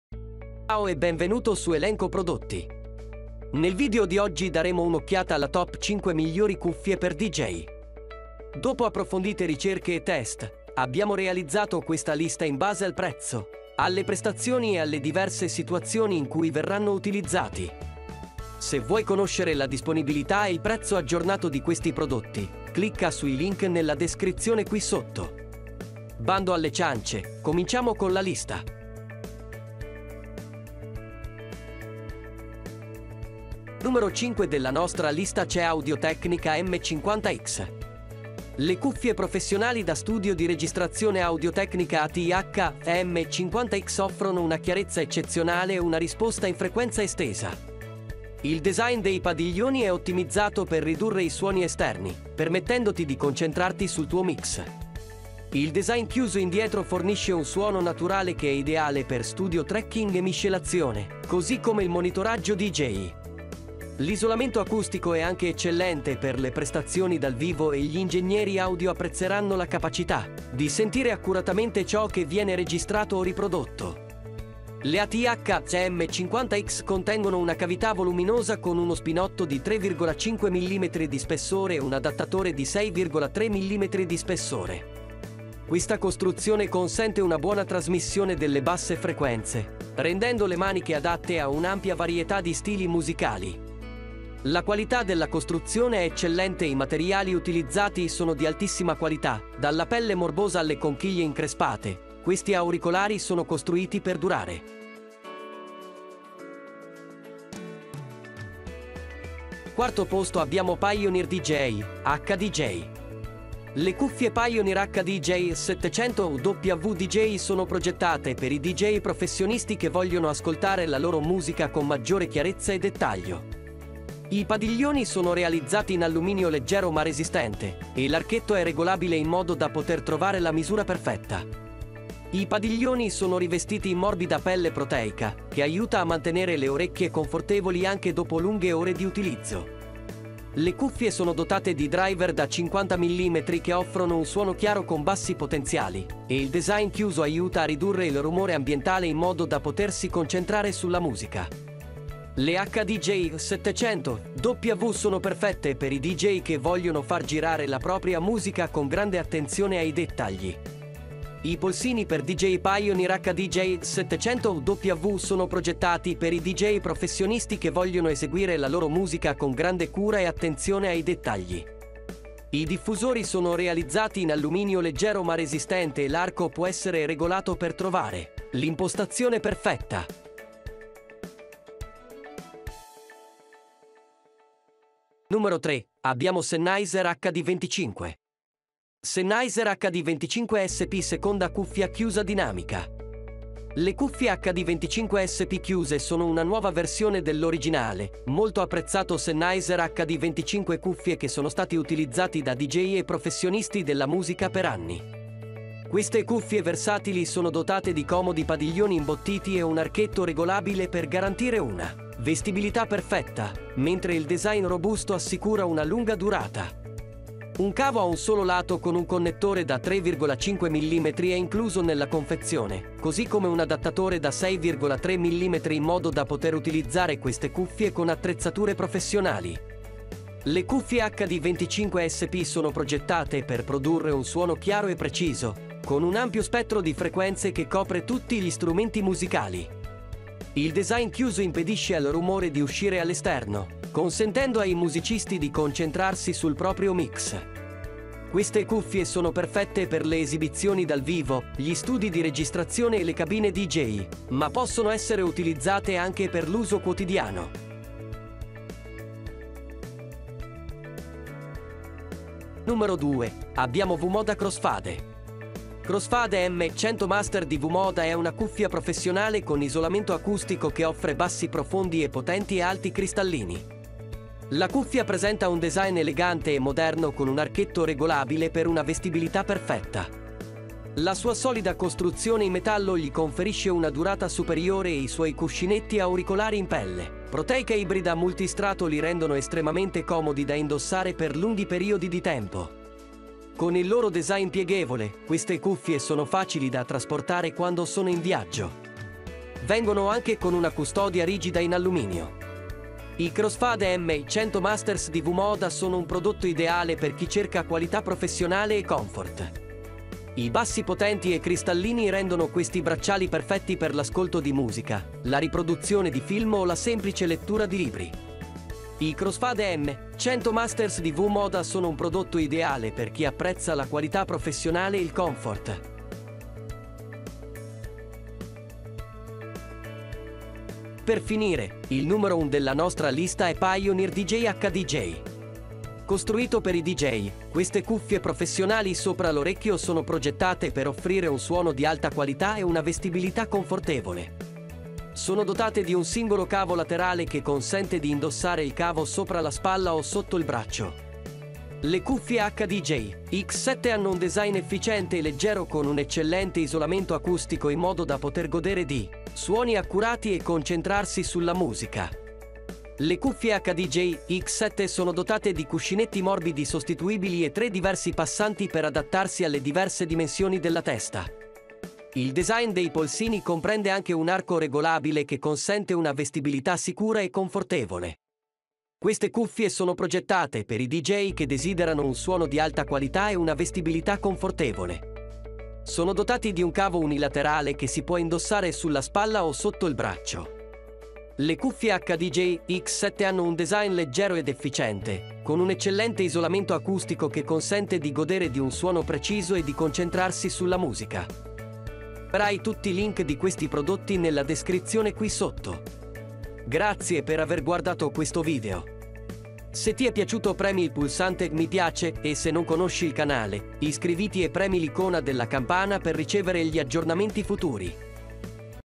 ⚡️Migliori Cuffie per DJ 2023
L'elenco di 5: Migliori Cuffie per DJ

1. Pioneer DJ HDJ

2. V-Moda Crossfade

3. Sennheiser HD 25

4. Pioneer DJ, HDJ

5. Audio-Technica M50x